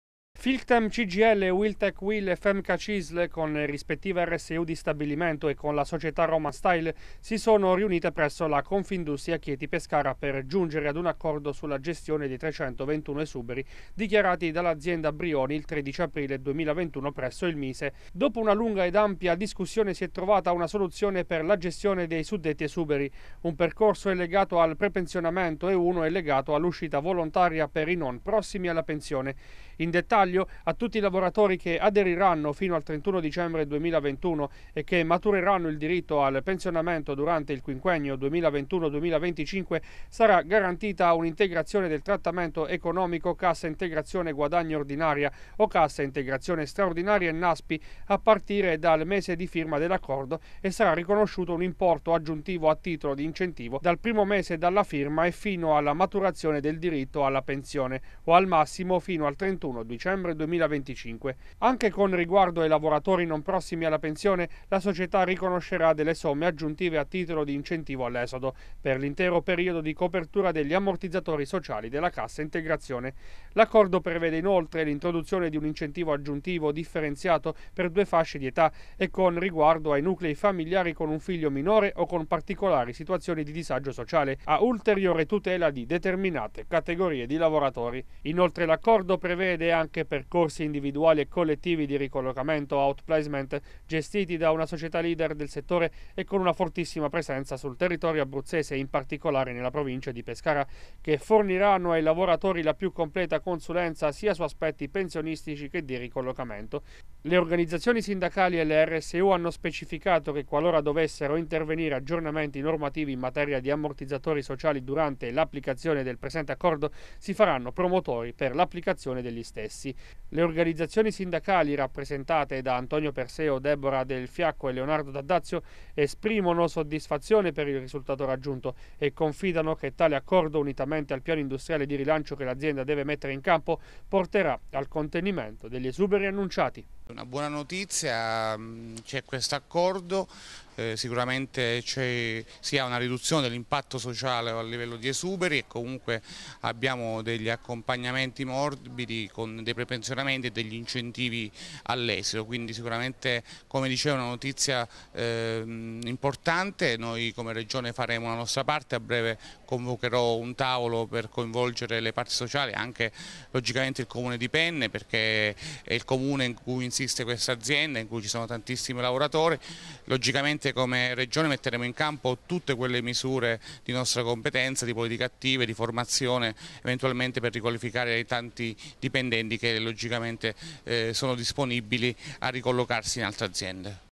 Brioni. Accordo quadro tra sindacati e azienda
Nell’ultimo incontro tra sindacati e la proprietà di Brioni, il marchio dell’alta moda maschile, è stato raggiunto l’accordo atto a perseguire il necessario riequilibrio economico-finanziario della Società cercando di contenere il più possibile l’impatto sociale ed occupazionale.